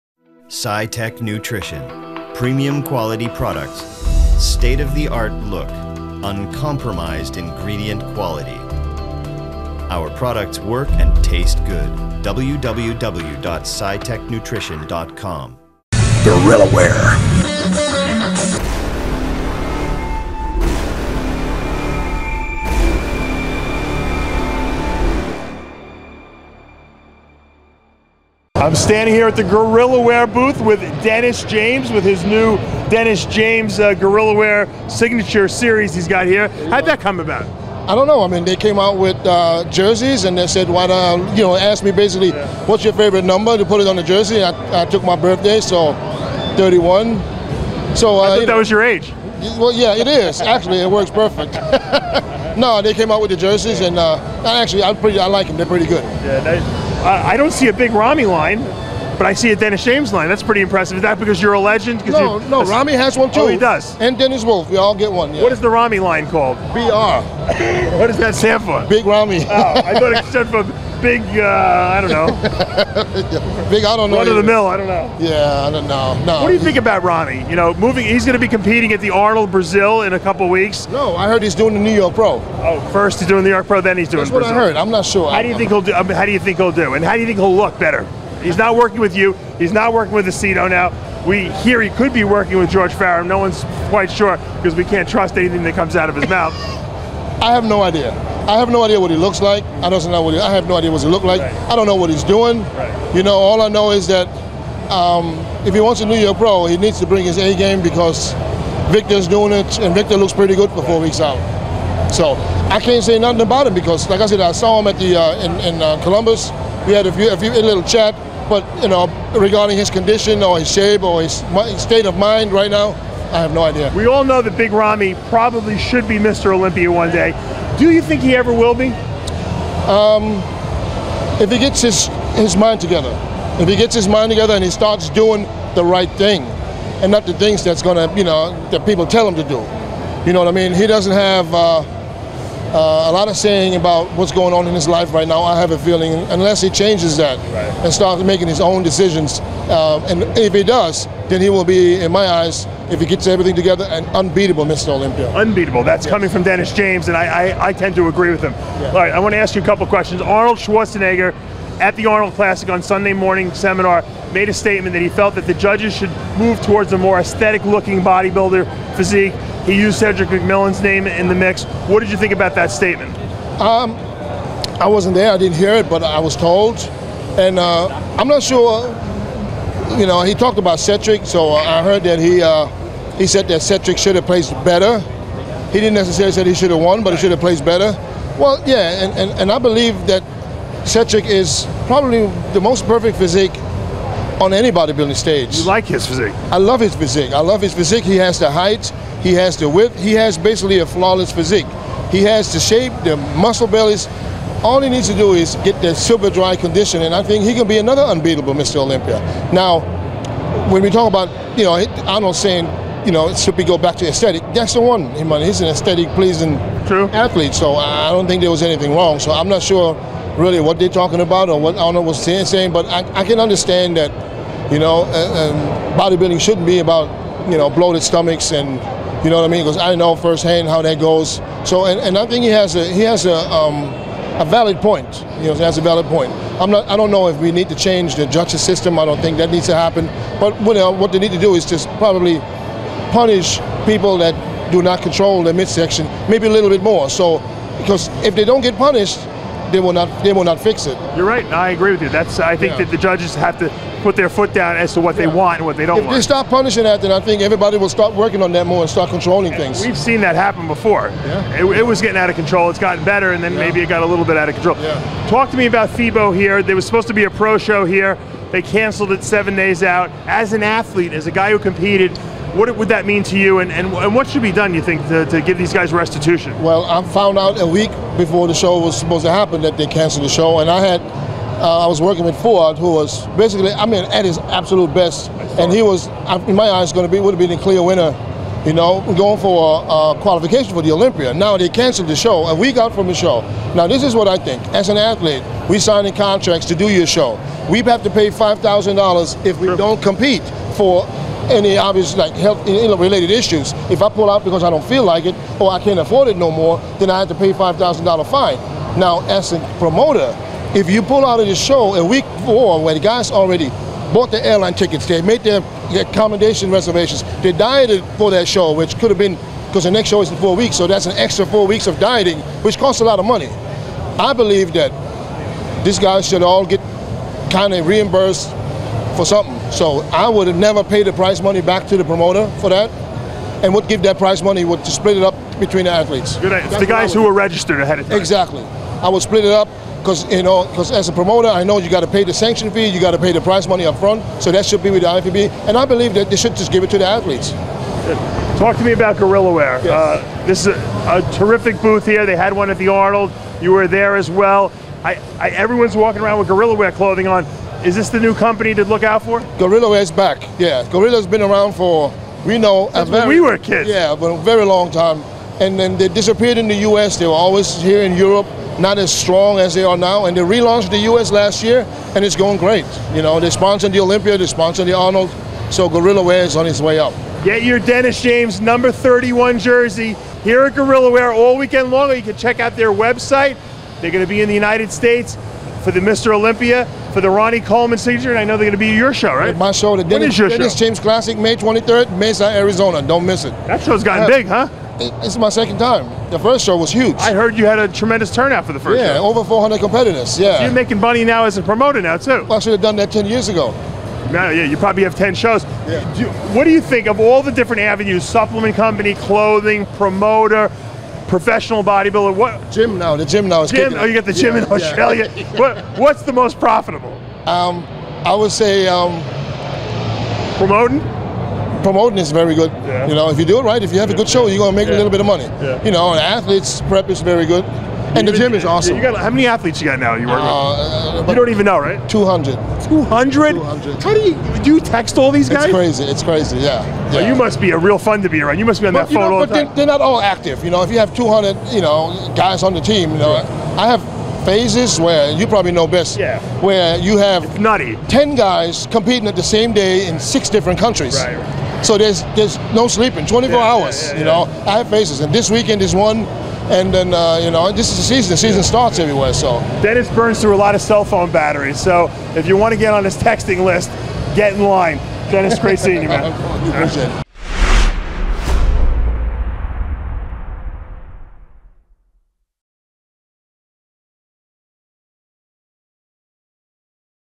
Dennis James Talks Bodybuilding at Gorillawear at FIBO 2015
Dennis James talks about the latest breaking fashions in Gorillawear.  Plus, he discussed the last minute cancelation of FIBO POWER 2015